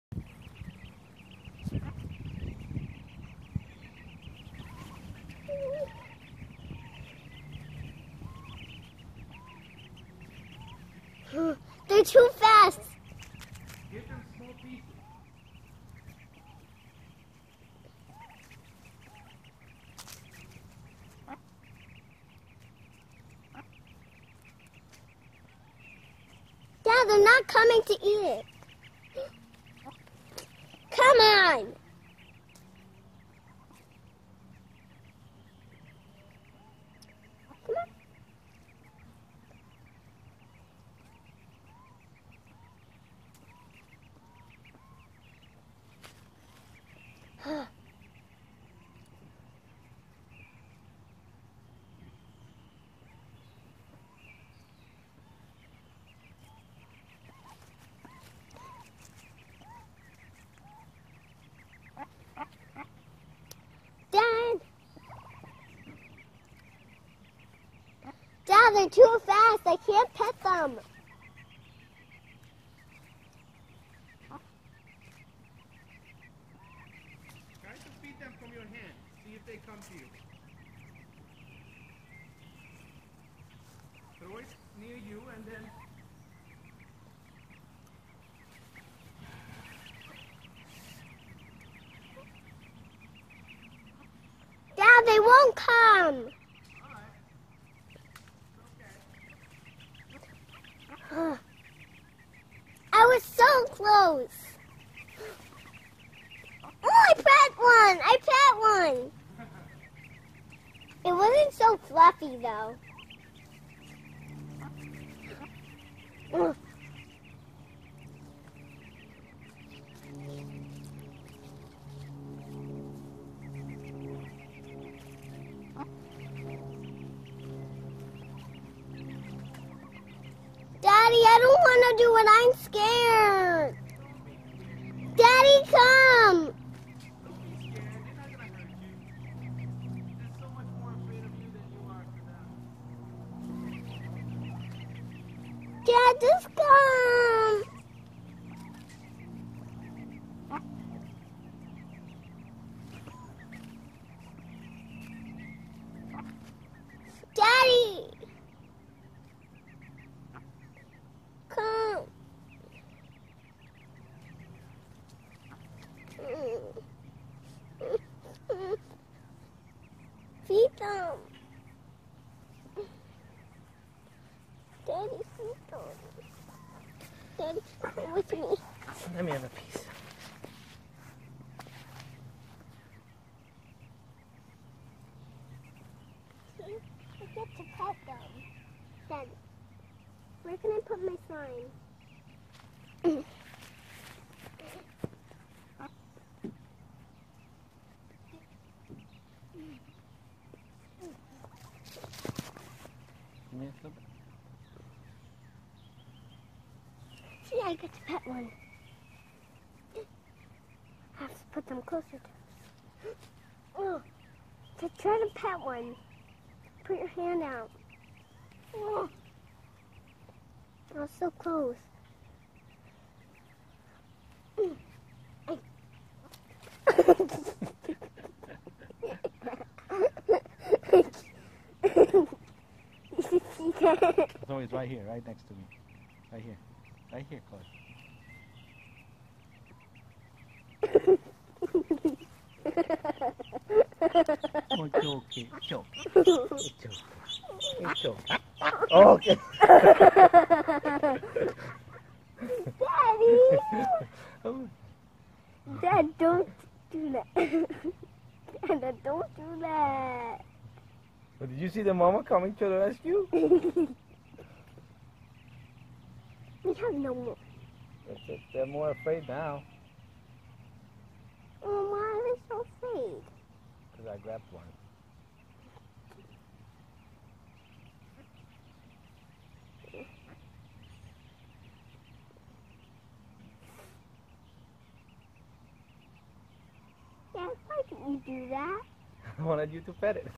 Try to steal a baby duck..
Try to steal a baby duck.. (fast forward to 3:41 to see the attempt)
FYI: we feed and pamper baby ducks and gosling in our back yard each year and the same family comes back year after year to our pond. No harm is ever done to them.

Timestamps:
0:00 Feeding and petting attempts
2:09 Daddy, I'm scared
3:42 Attempt to pet one.
4:00 Successful, Mama Rescue